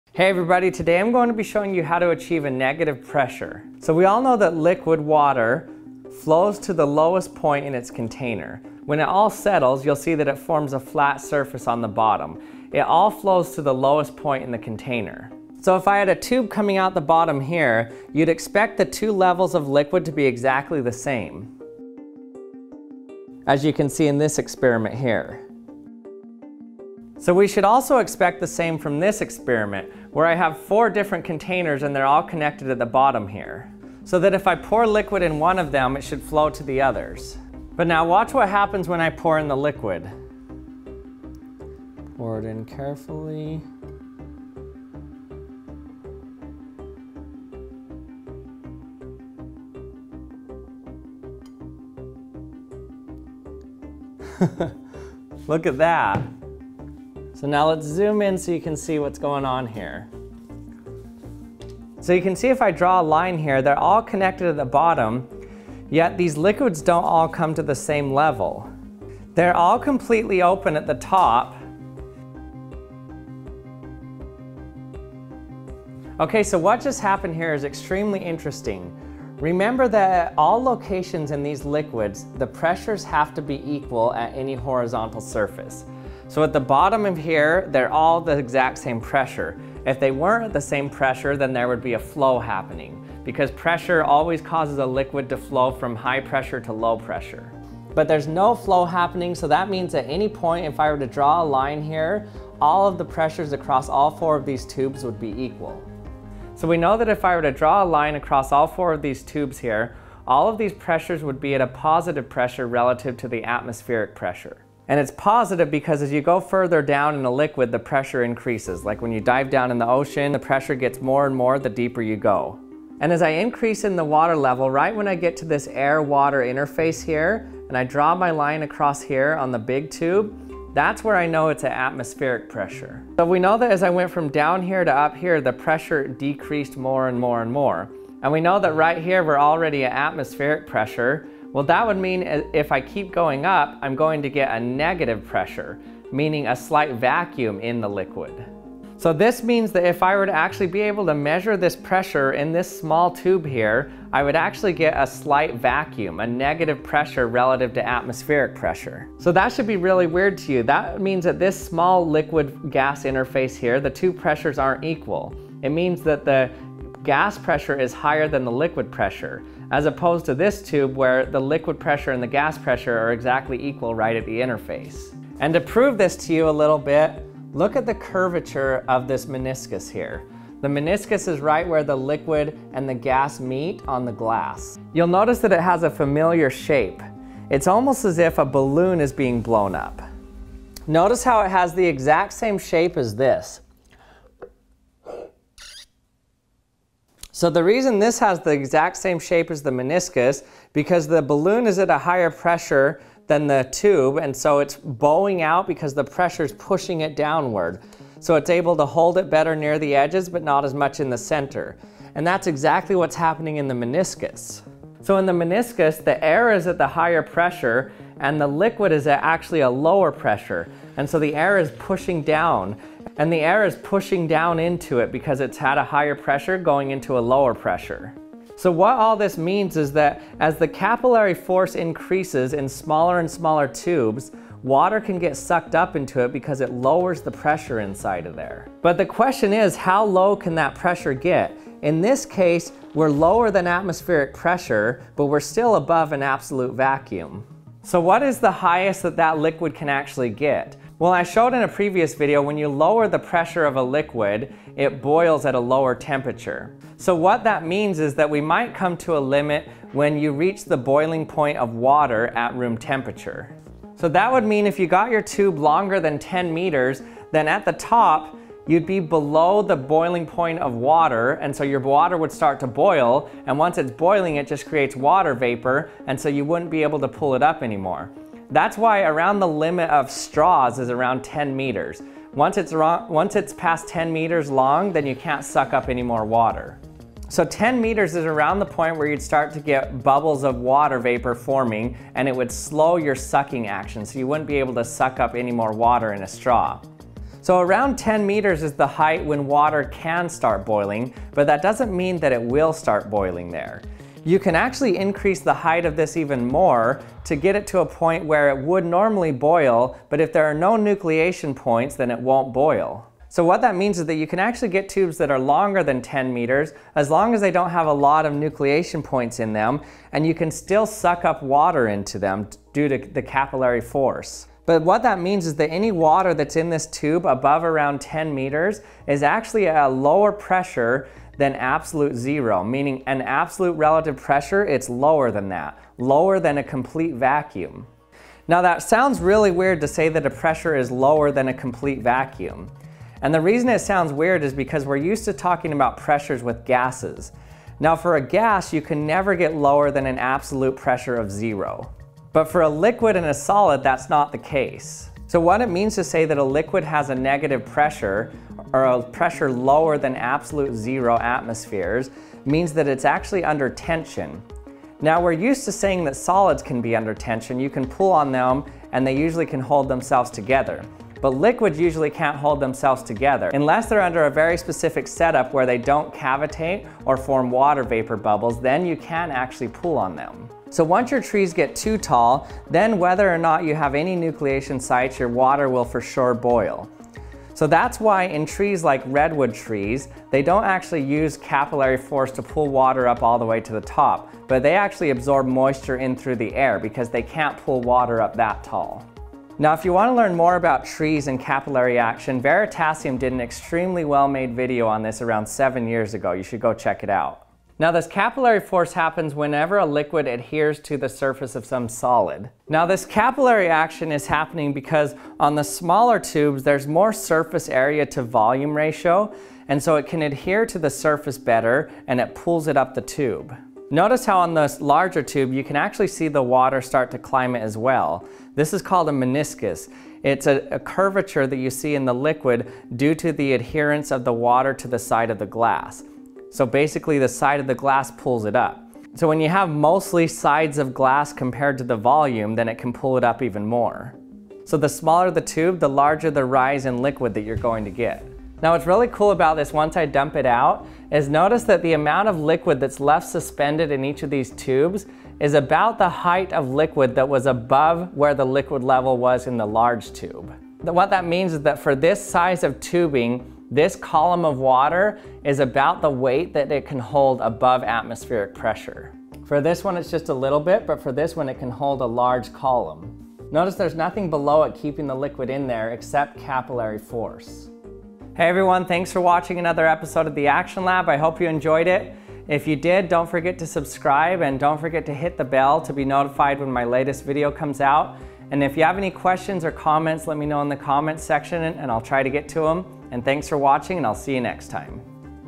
How to Make Low Pressures with a Capillary tube
In this video I show you how capillaries can decrease the pressure in a liquid well below an absolute vacuum if they are small enough